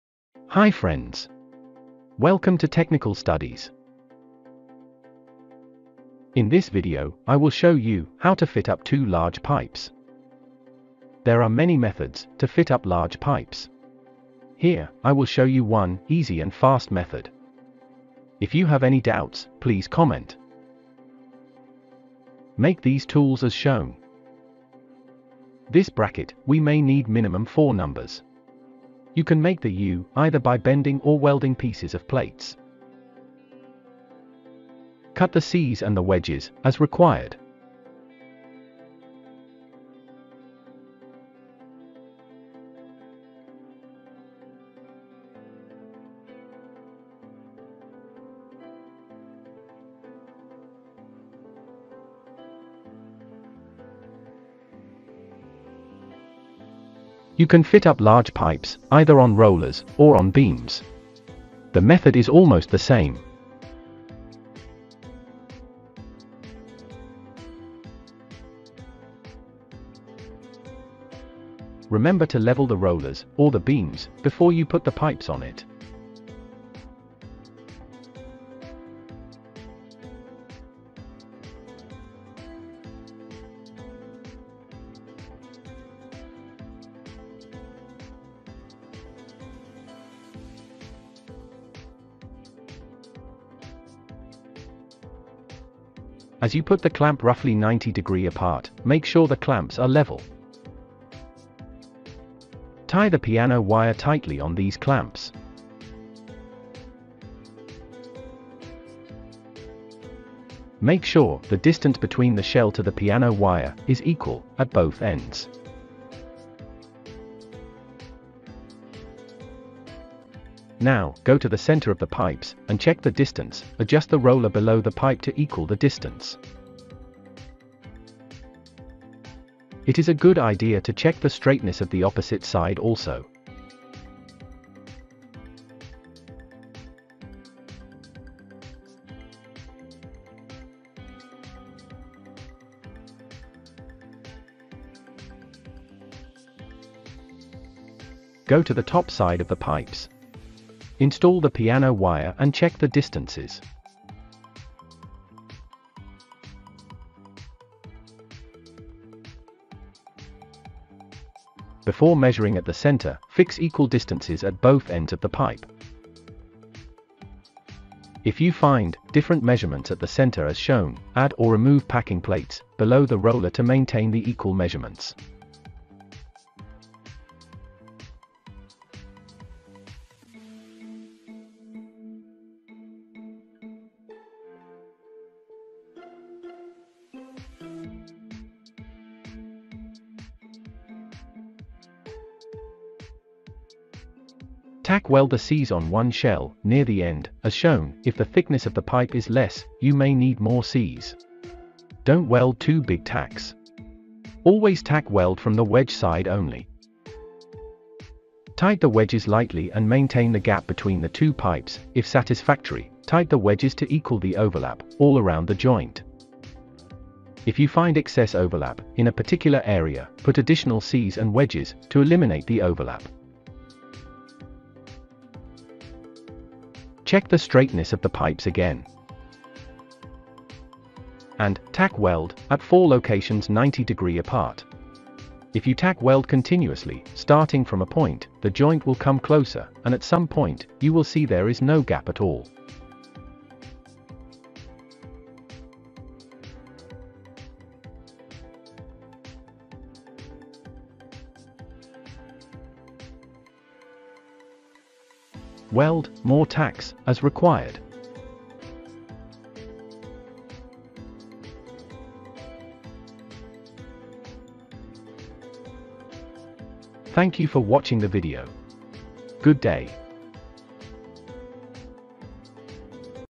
How to join large bore pipes
How to fit up two large pipe together, easy, fast , step by step method for beginners.  @technicalstudies.